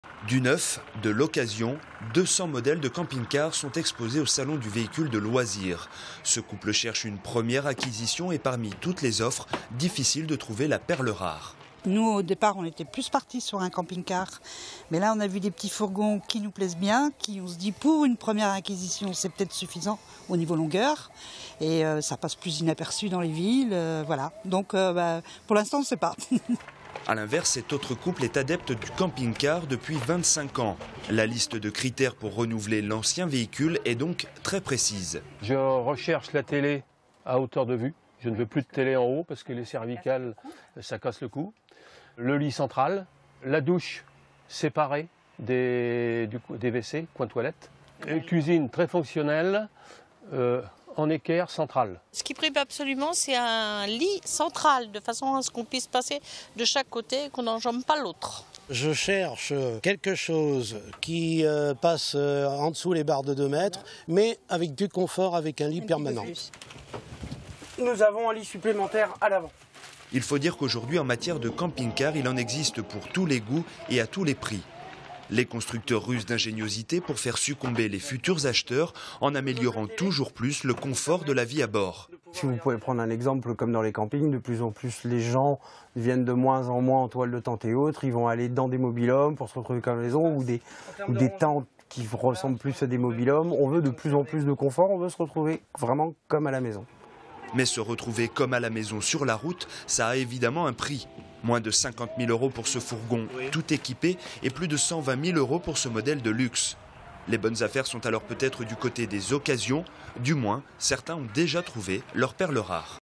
Véhicule de loisirs : Les tendances pour les camping-cars
Sites webs/blogs, copiez cette vidéo avec le bouton "Intégrer" !
 Direction le salon du véhicule de loisirs de La Roche-sur-Yon pour faire le point sur les nouvelles tendances en matière de camping-car.